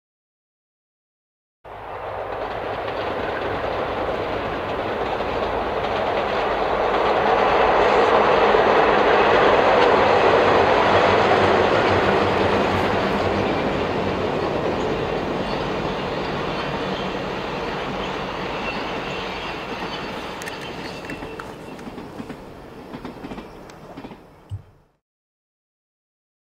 UP951 East - Hammett, ID
With nary a signal other than green, UP951 has the passenger special zipping along through Hammett, ID on 07/15/2012 at 14:53 (making it a wee bit difficult for me to get ahead and setup).  In a few miles, the special will slide over to #1 at the east end of Glenns Ferry to get ahead of traffic ahead.

See a few other captures of the UP951 and consist with the UP 150th special returning to Cheyenne after the stop in Portland: